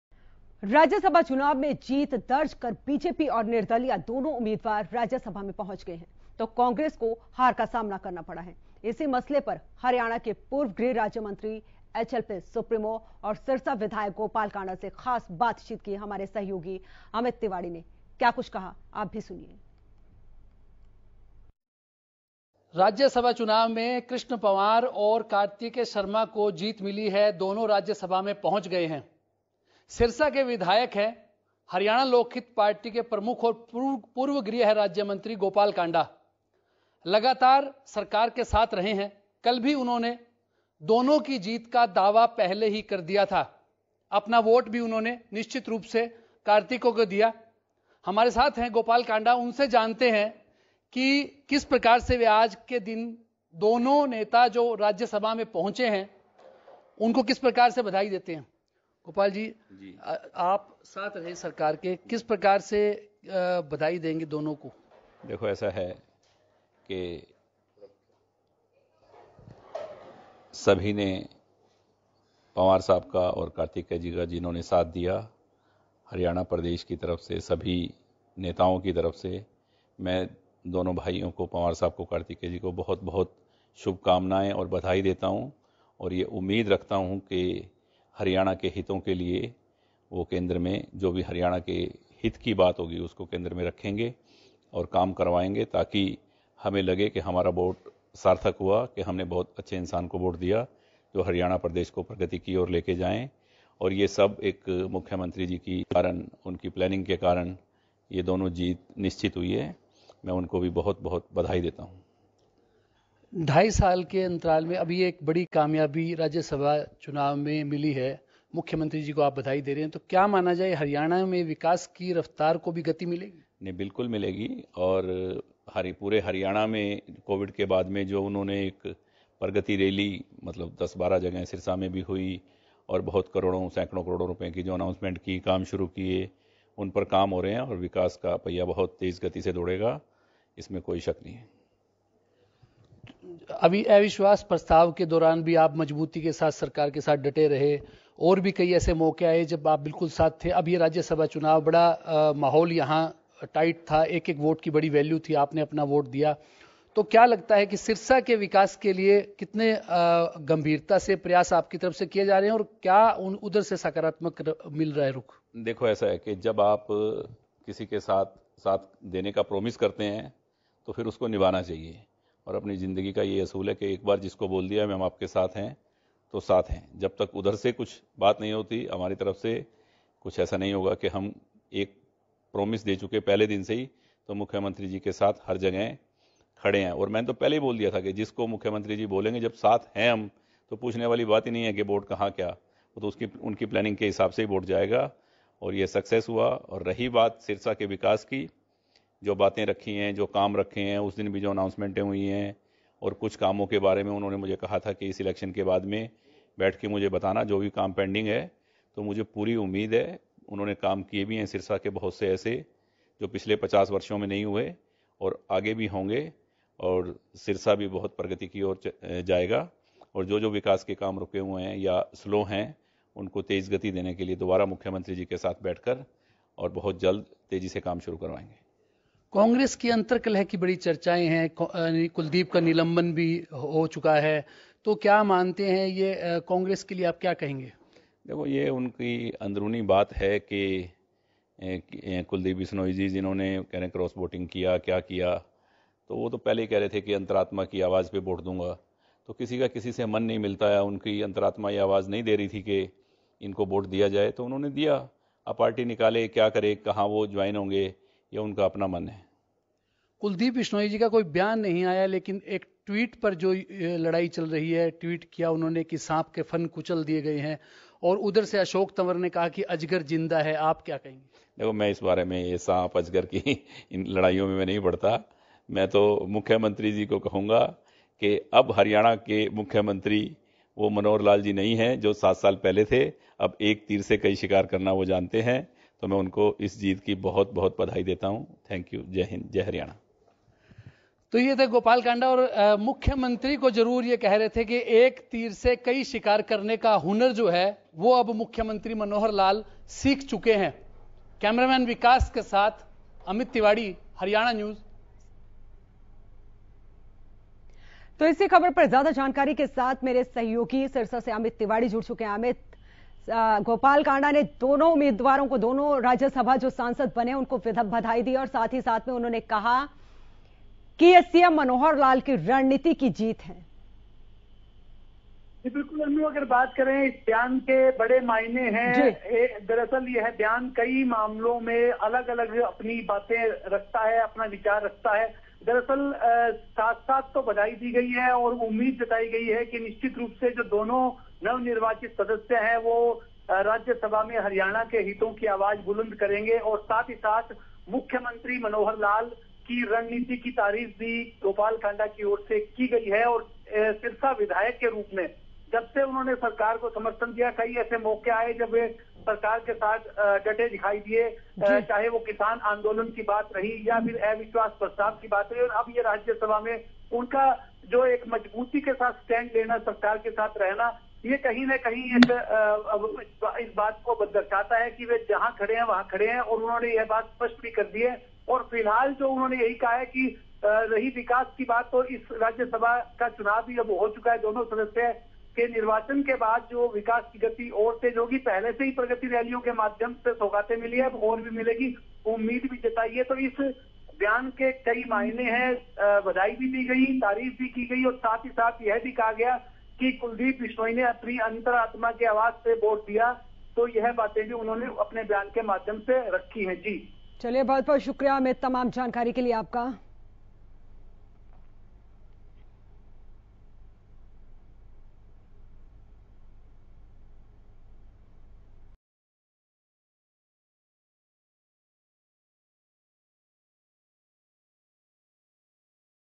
विधायक Gopal Kanda ने बीजेपी-निर्दलीय उम्मीदवार को दी जीत की बधाई, सुनिए क्या कहा?
राज्यसभा चुनाव में जीत दर्ज कर बीजेपी और निर्दलीय दोनों उम्मीदवार राज्यसभा में पहुंच गए हैं. तो कांग्रेस को हार का सामना करना पड़ा है.